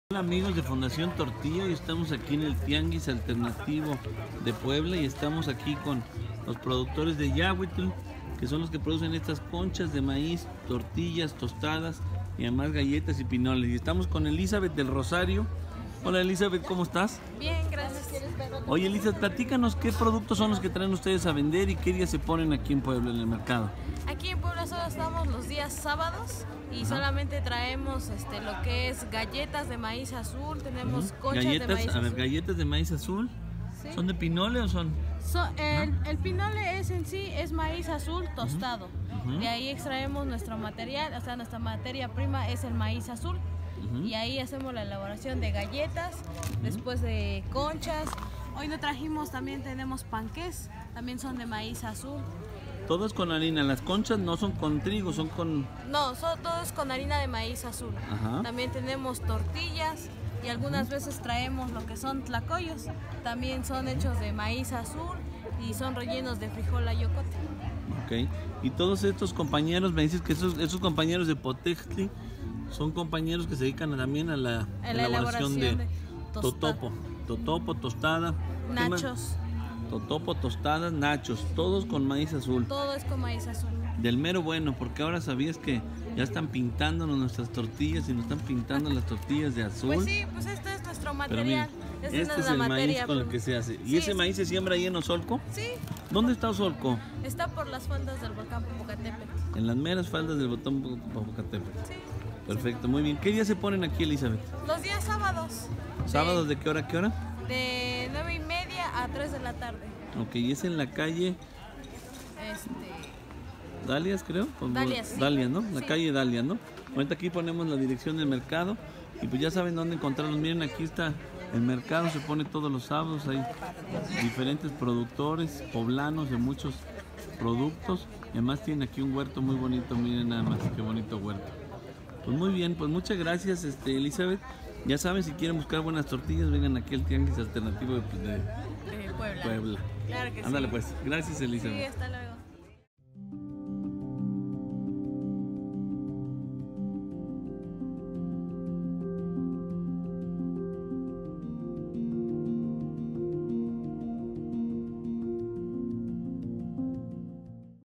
Conchas, tortillas, tostadas y galletas de maiz azul
¿Han probado las conchas de maíz?, pues bien, este es uno de los ricos platillos que nos presenta nuestra amiga Elizabet del Rosario de Yahuitl Ozolco, elaborados con maíz azul, podemos encontrar galletas, panqués, tortillas y más en el Tianguis Alternativo de Puebla. ¿Cuéntanos que otros panes de maíz has probado?

Este video fue realizado gracias al apoyo de Fundación Dondé.